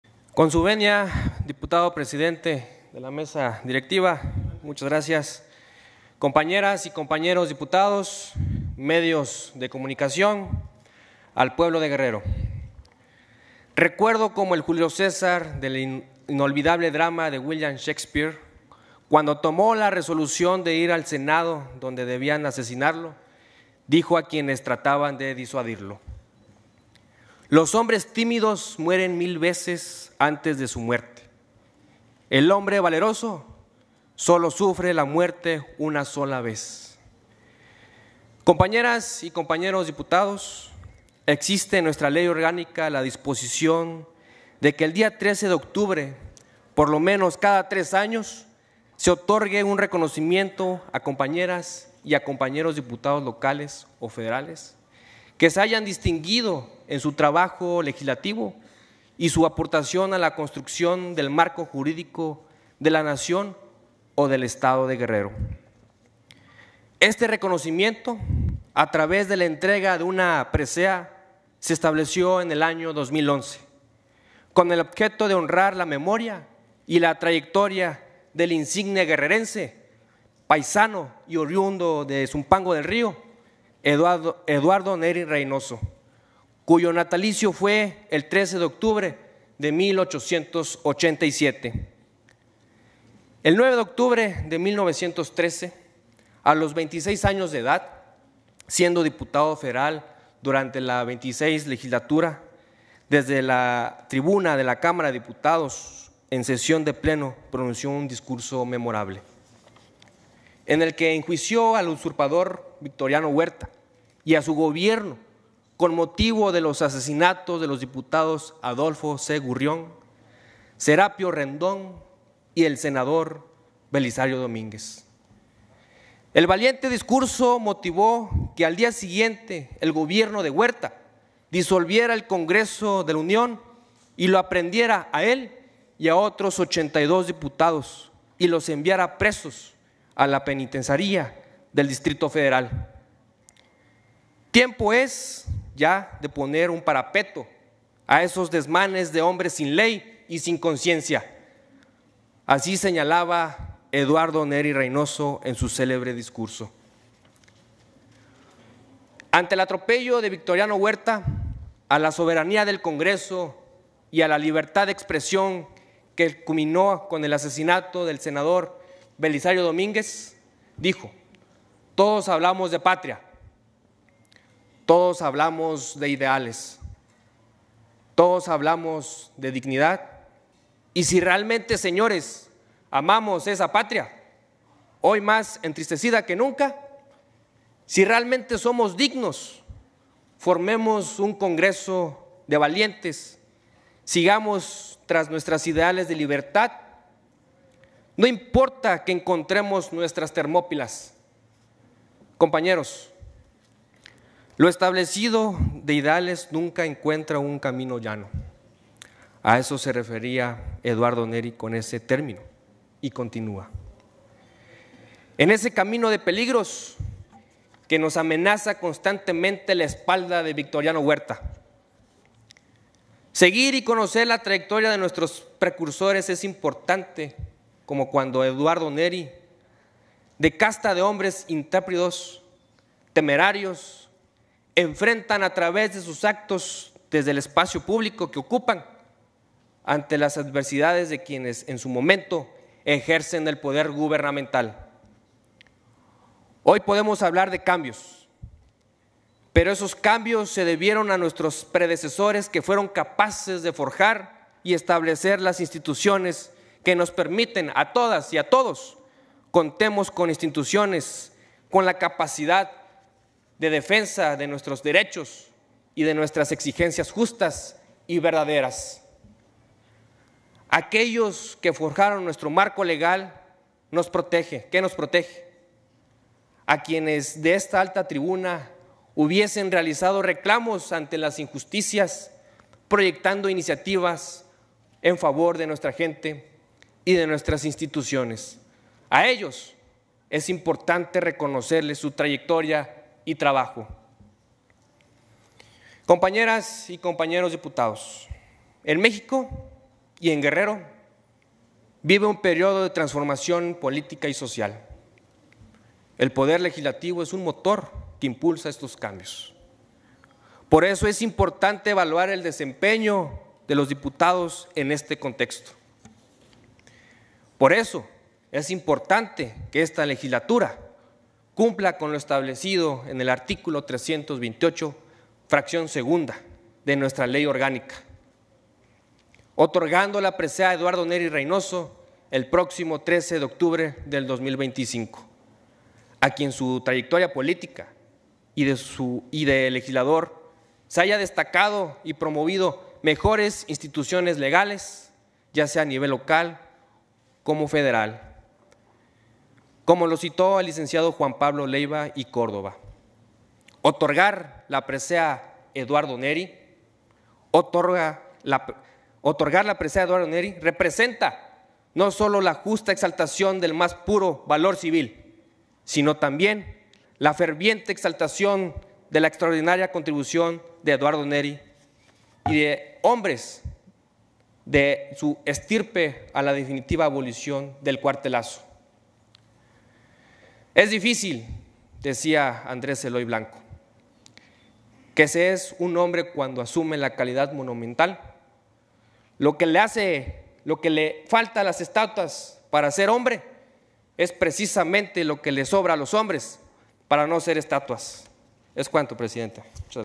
Proponen retomar la entrega de la Presea "Eduardo Neri Reynoso" en 2025
El diputado Jorge Iván Ortega Jiménez intervino para proponer la entrega de la presea "Eduardo Neri Reynoso" el próximo 13 de octubre de 2025, reconocida en la Ley Orgánica del Poder Legislativo de Guerrero. Resaltó que, en este periodo de transformación política y social, es crucial evaluar el desempeño legislativo y reconocer a quienes hayan impulsado mejoras en las instituciones.